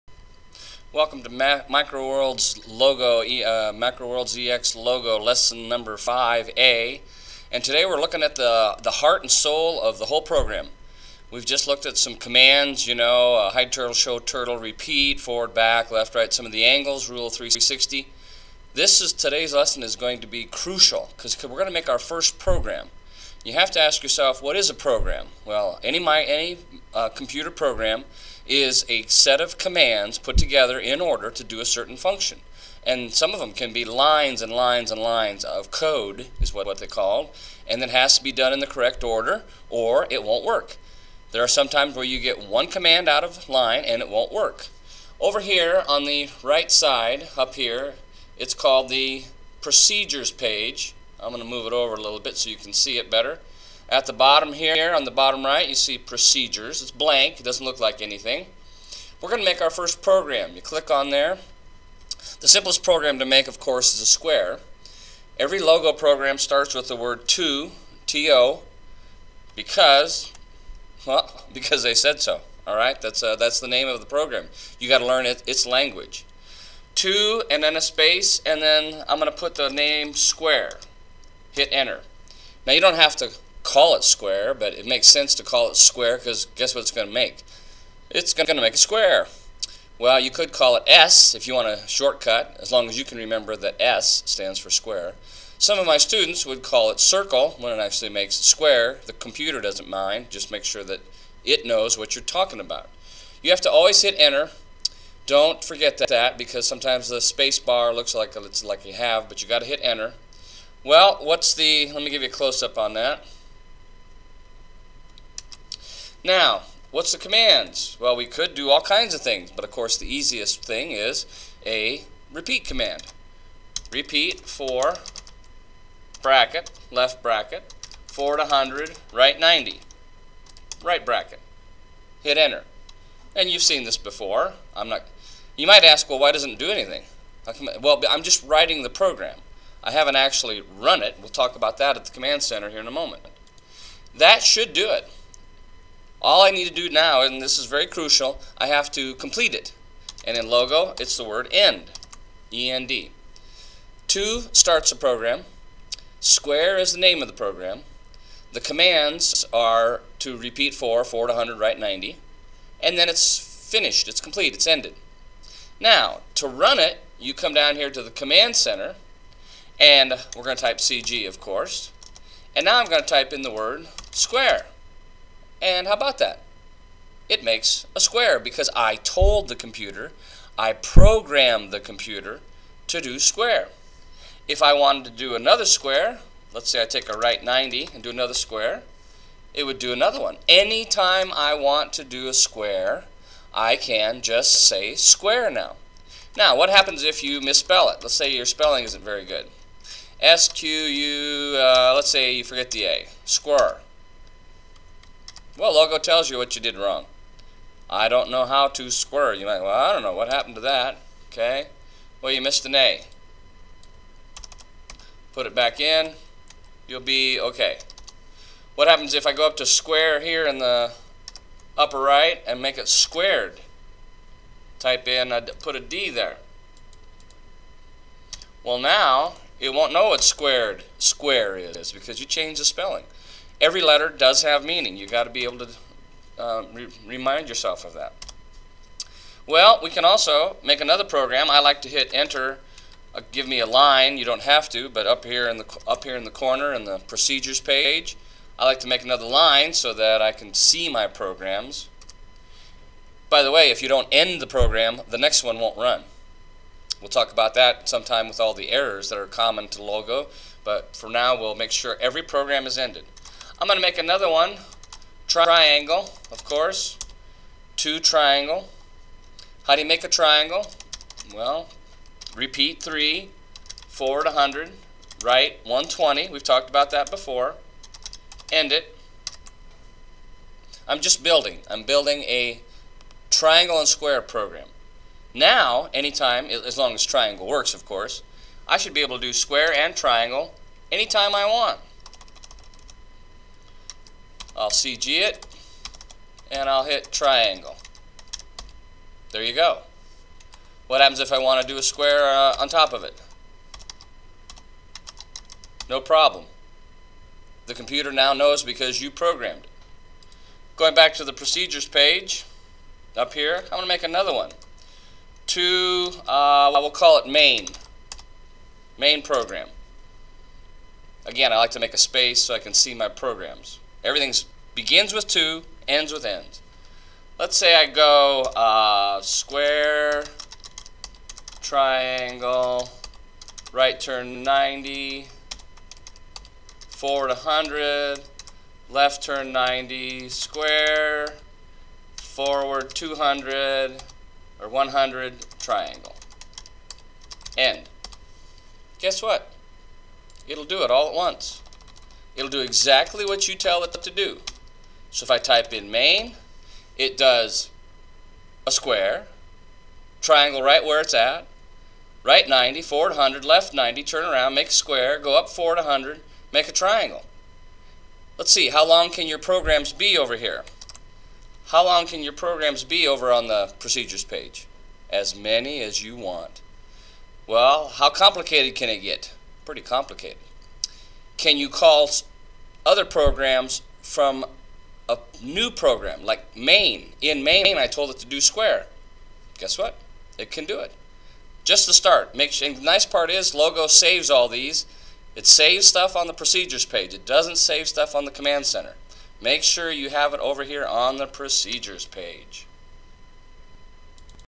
MicroWorlds EX Logo Lesson 5A
This is the companion video to Lesson 5 and it shows the screen shots to see the commands.  The first programs are built on the Procedures page.